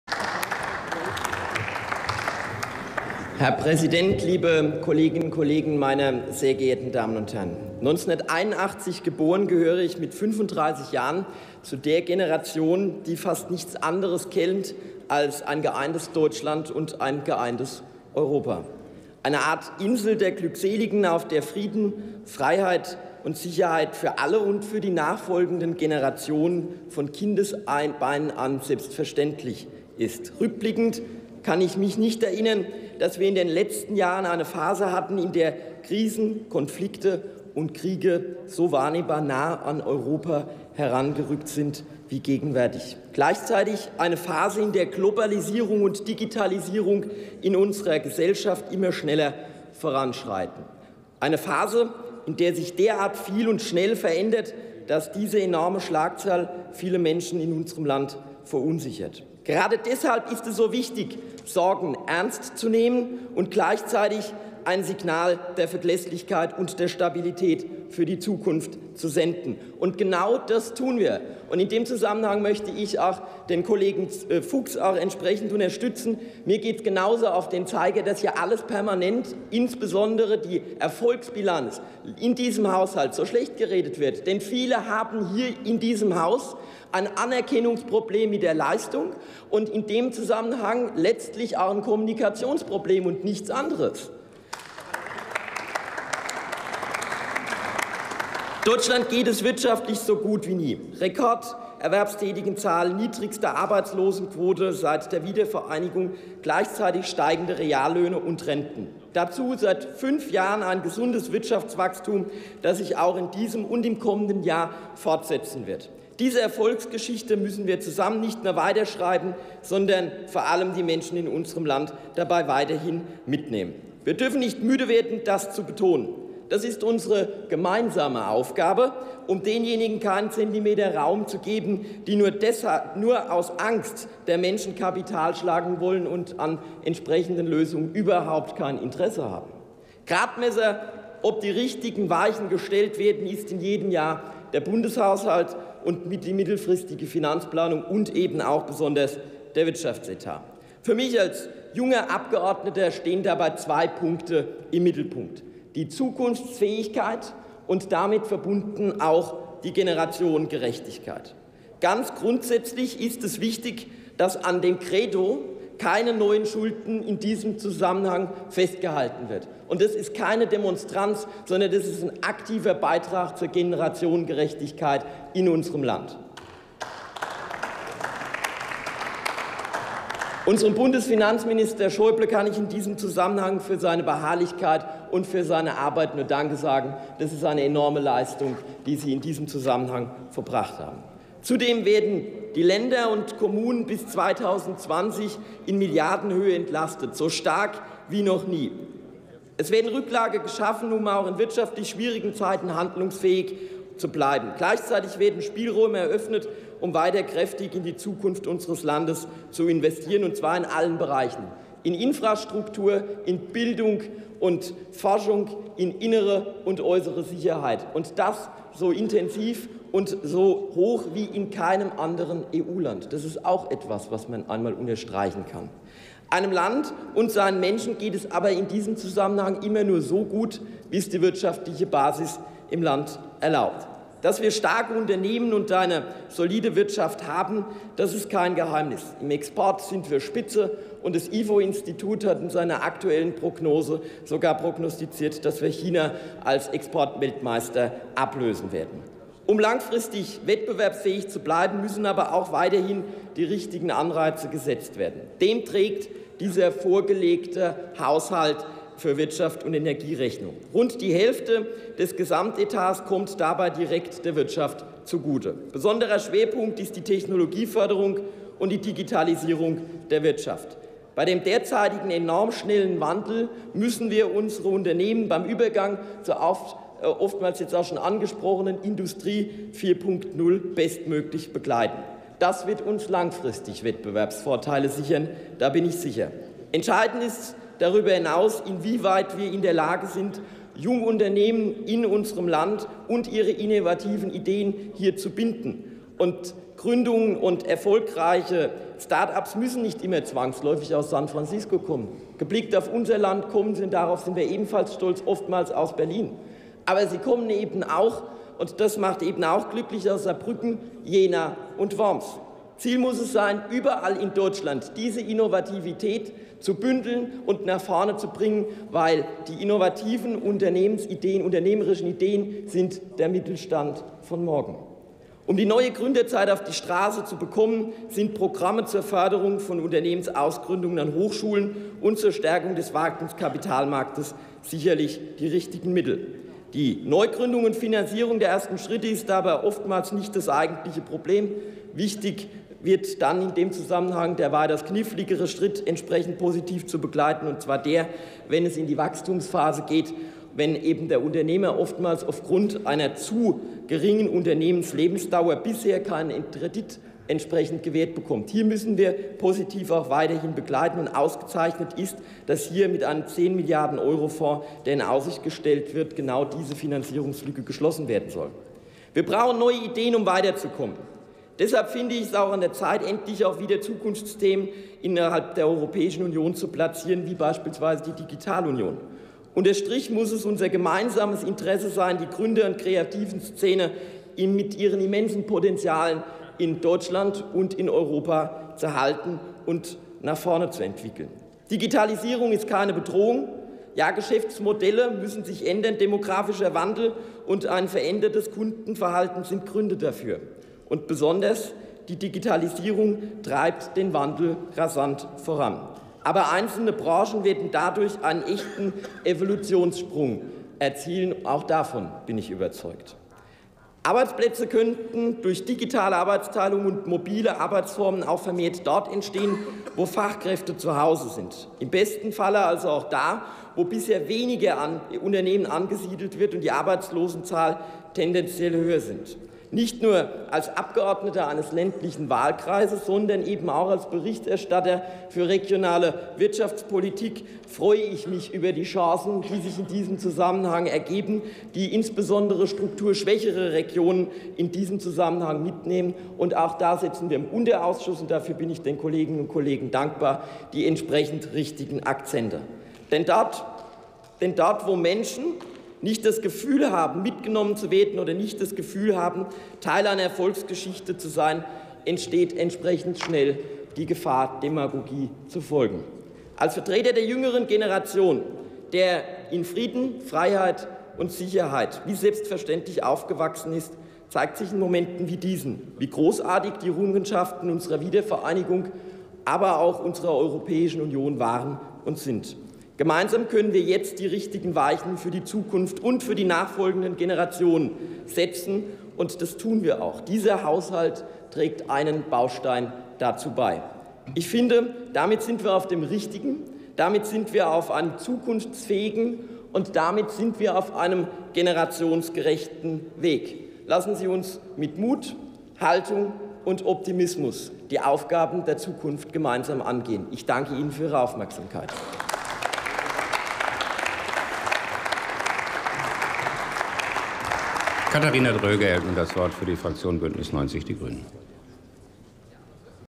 Jan Metzler: Wirtschaft und Energie [Bundestag 08.09.2016]
Erste Beratung der Bundesregierung
Einzelplan 09 (Wirtschaft und Energie)

18. Deutscher Bundestag
187. Plenarsitzung
Tagesordnungspunkt: Wirtschaft und Energie
Redner: Metzler, Jan, CDU/CSU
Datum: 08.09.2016
Plenarprotokoll:
UVID:6999657